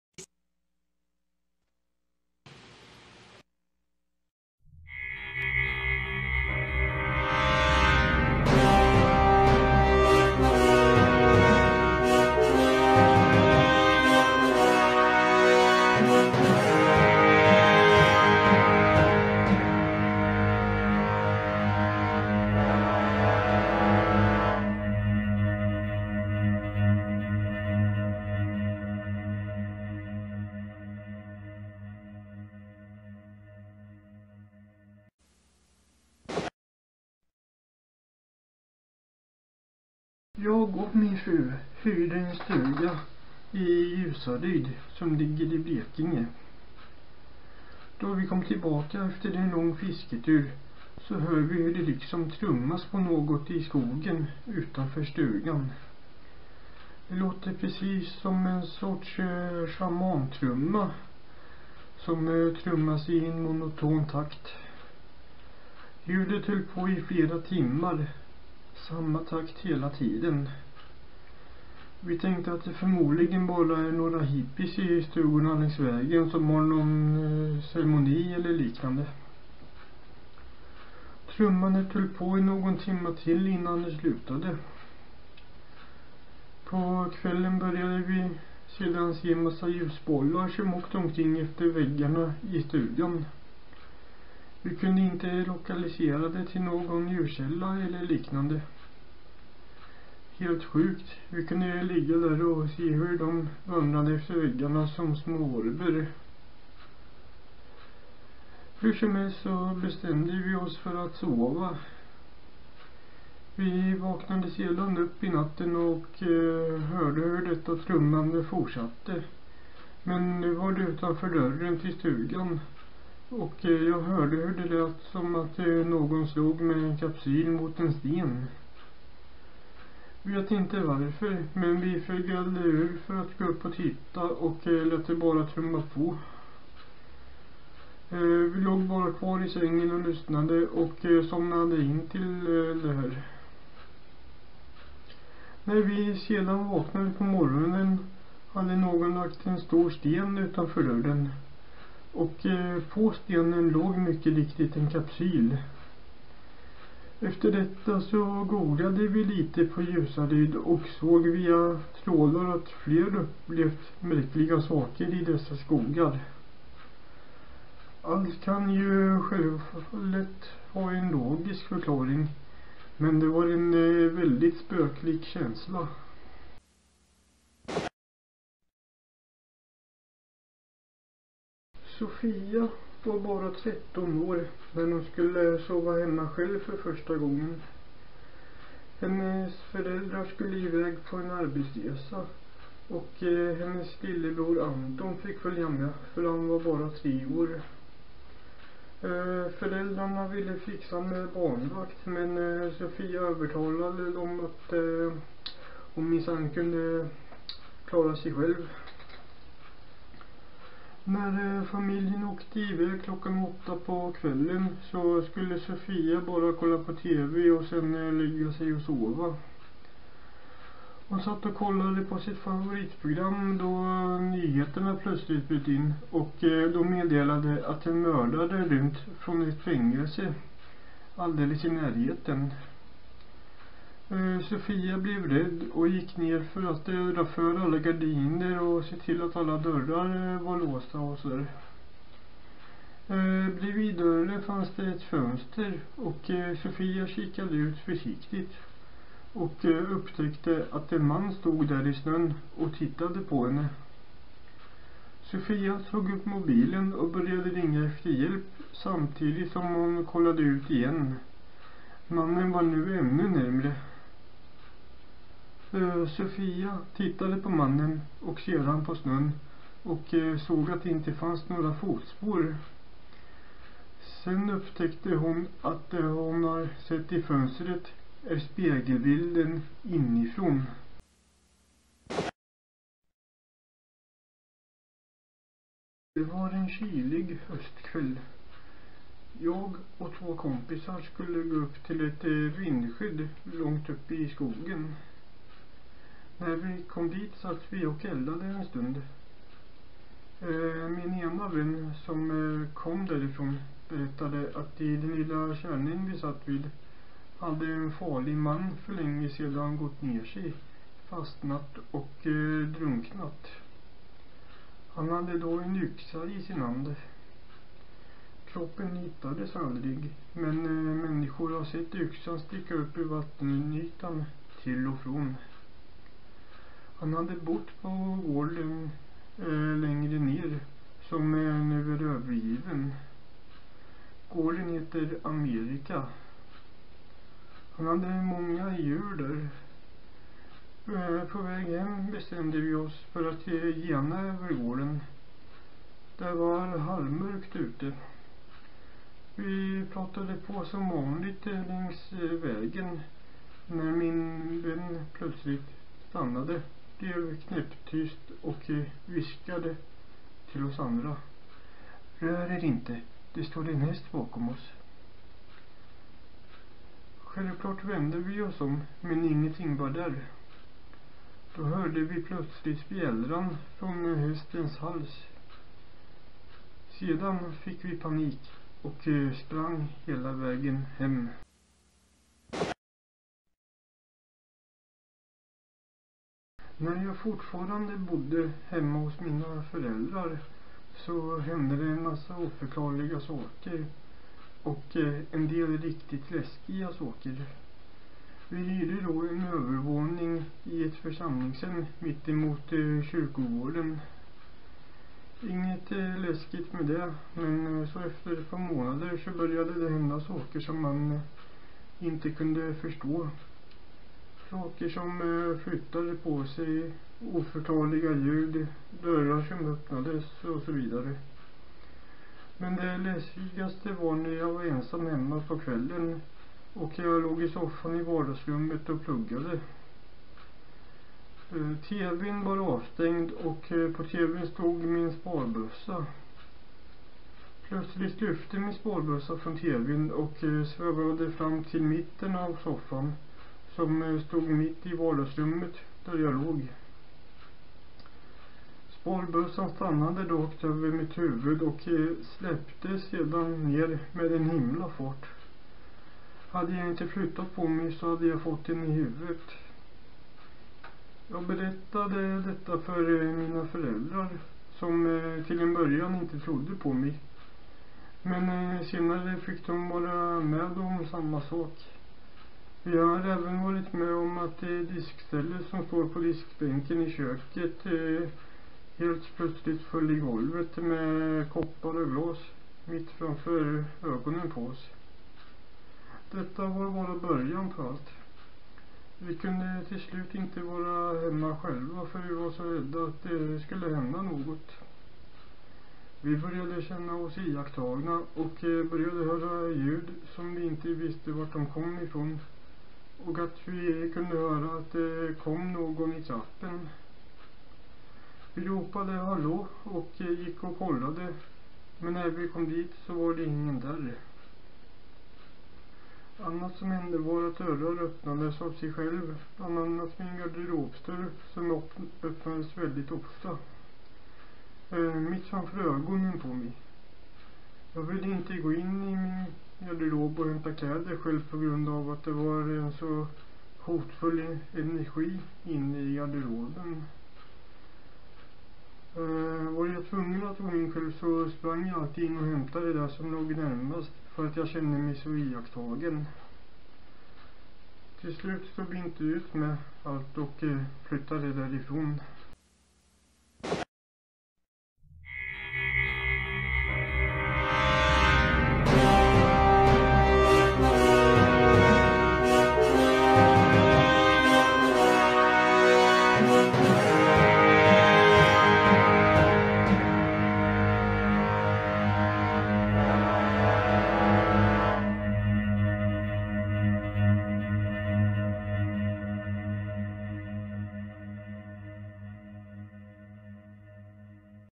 Motalamannens spökhistorier: Del 2 - Korta spökhistorier
Det är tätt mellan varven - andra avsnittet släpps redan nu! Här bjuder jag på fyra korta spökhistorier.